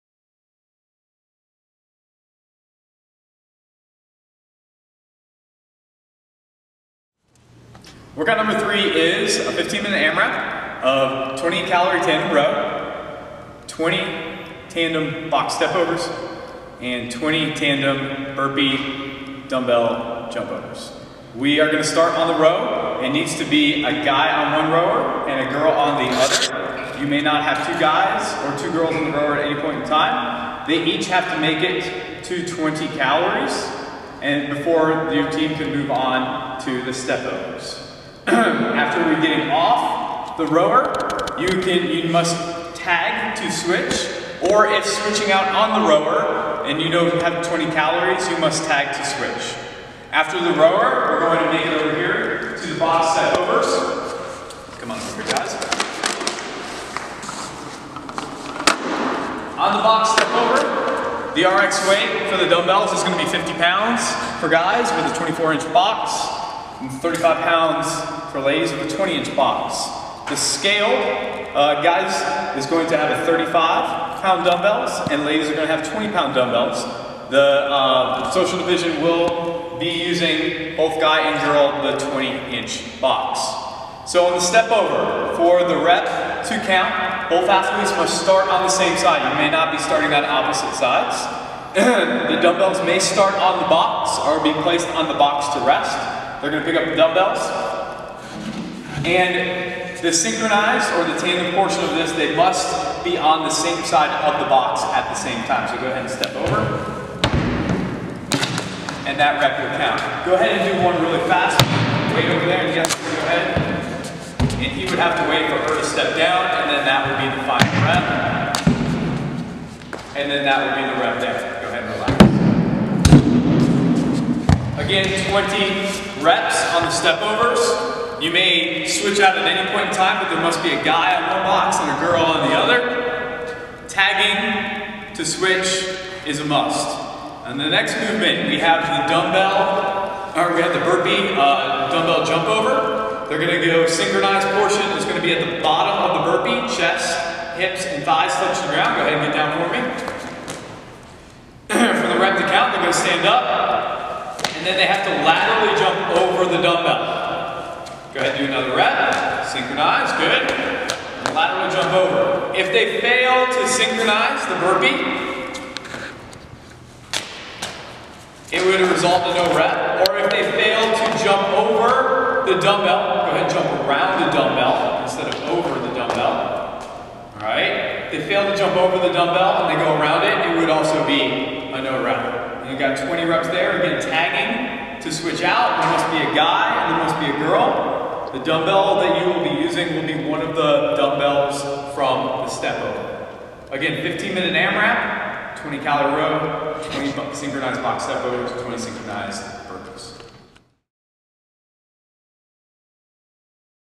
Birthday Bash WOD 3
15-minute AMRAP
Tandem row x 20 cal
Tandem box step over x 20 (50/35, 24/20; 35/20, 20)
Tandem burpees over dumbbell x 20

Notes:
• One male and one female will be working the entire time.
• You may switch teammates whenever you want, but you can’t have two males or two females working at the same time.
• Tandem burpee will have both athletes chest on the ground at the same time and they must jump over the handle of the dumbbell.
• Tandem box step ups will have a complete rep when both athletes have stepped over the box and have both feet on the ground.